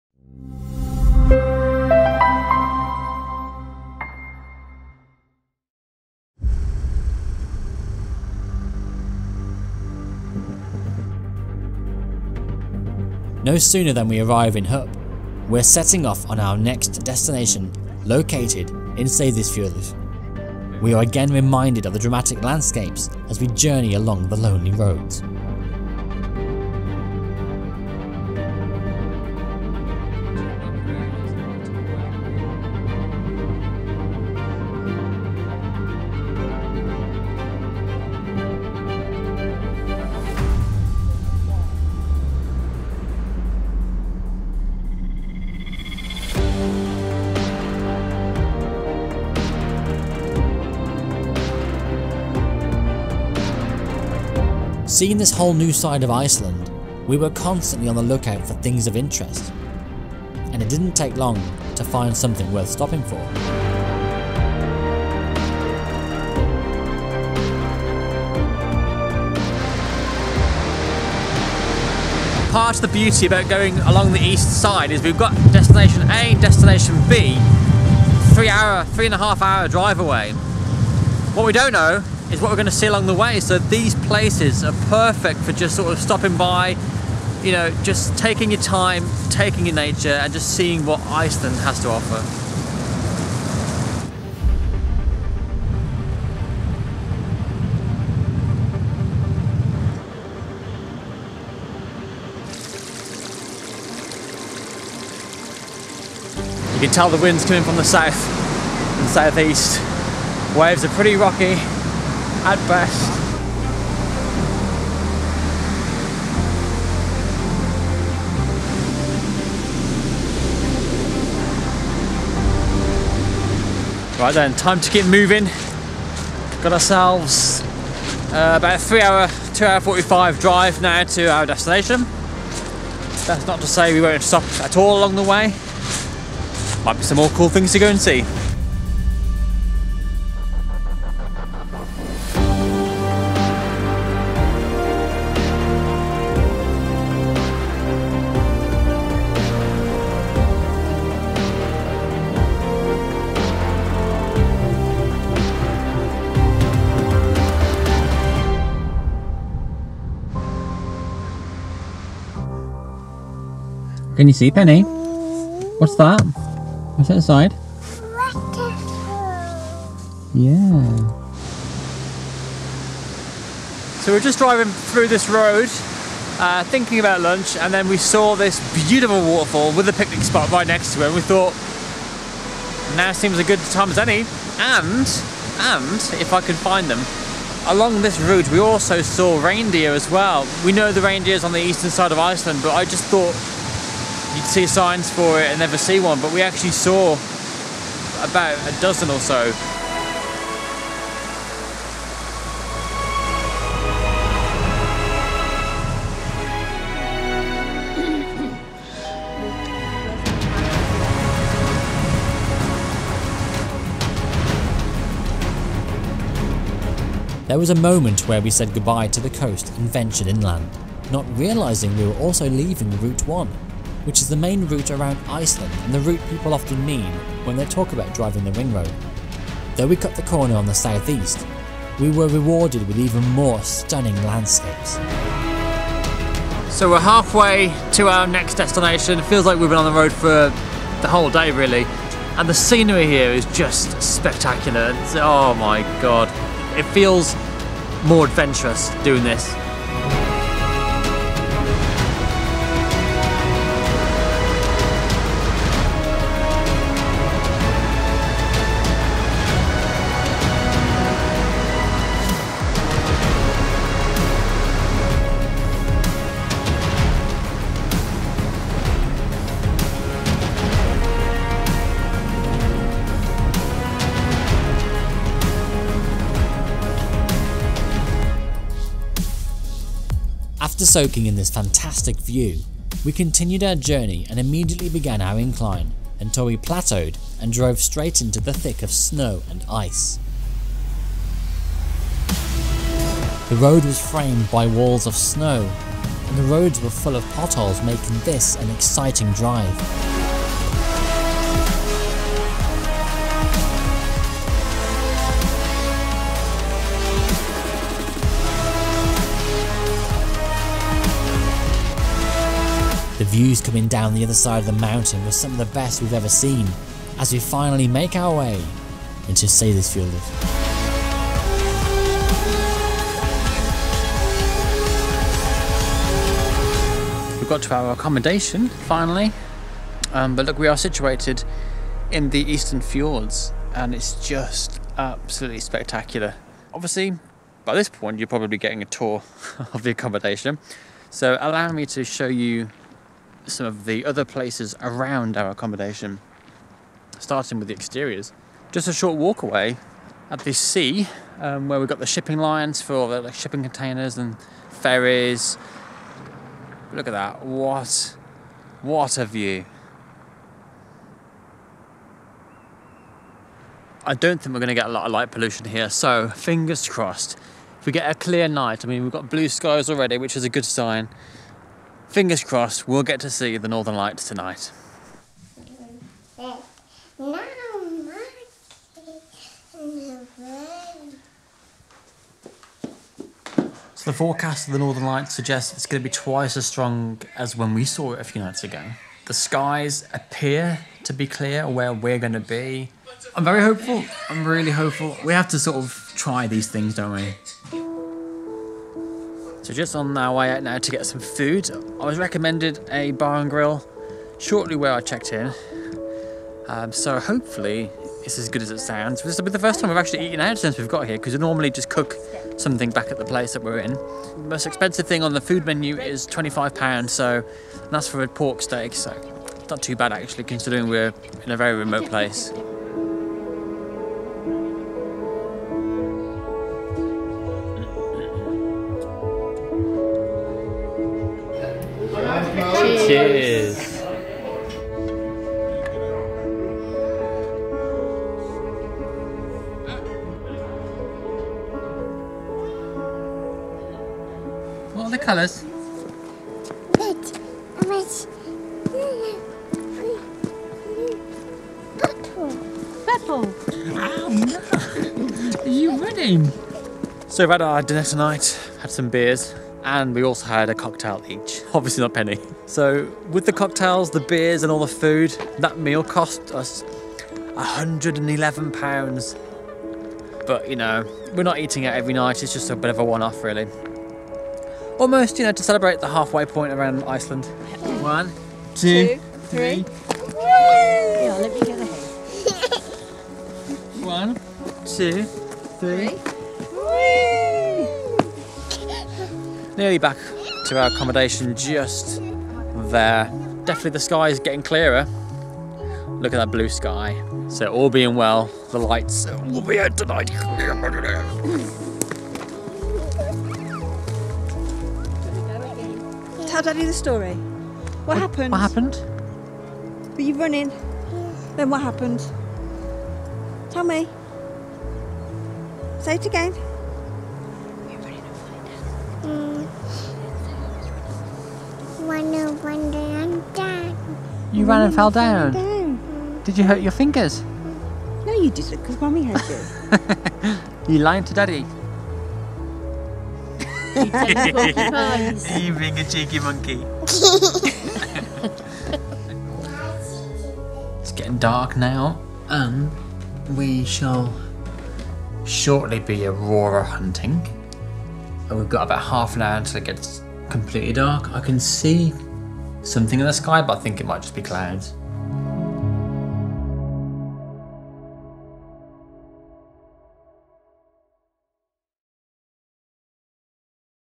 The BEST views in Iceland? ICELAND VLOG 06
We leave Hofn for the eastern town of Seydisfjordur. Along the way, we stop by a beautiful black sandy beach, a gorgeous waterfall in the vast mountains and navigate our way through the snow and ice of these unfinished roads.

Seydisfjordur is a beautiful town with bright colours, a lovely charm and friendly people. We would highly recommend coming here. The scenery is just spectacular!